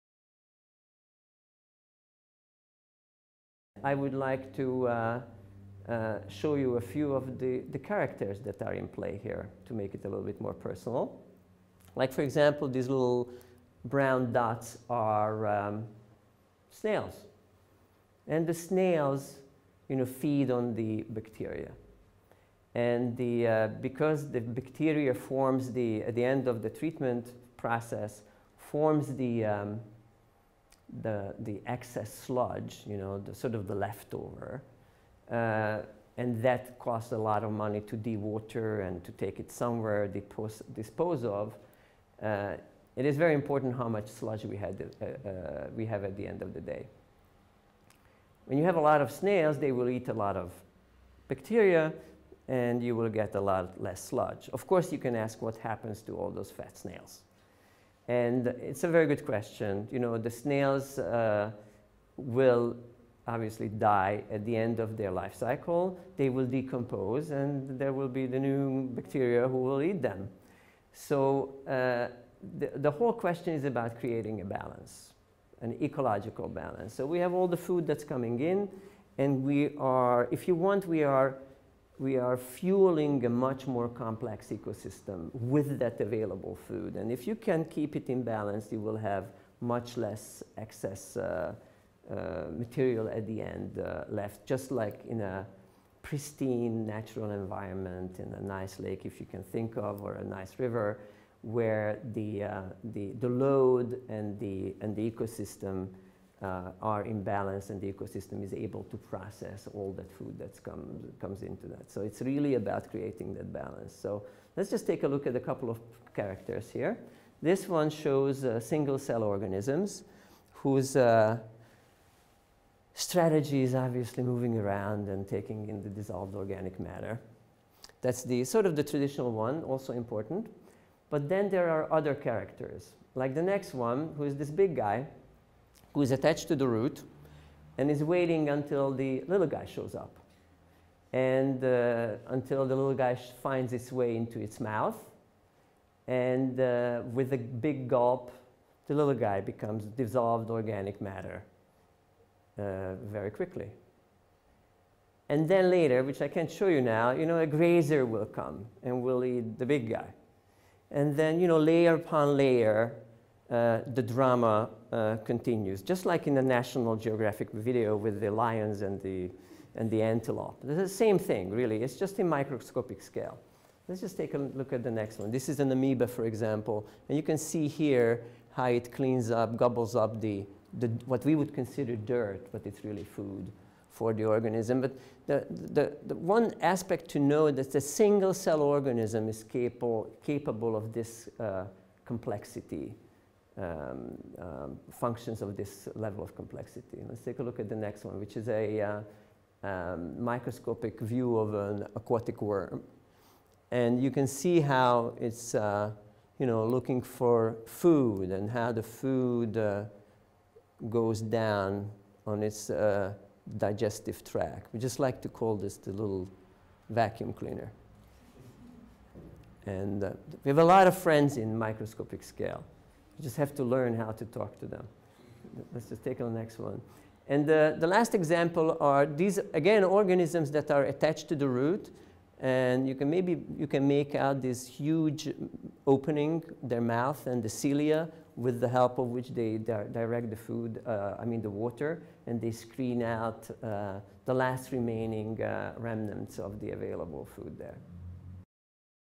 Organica Water Webinar - 6 Drama under the microscope
Examples of different survival strategies -- Predation under the microscope -- Microorganisms in action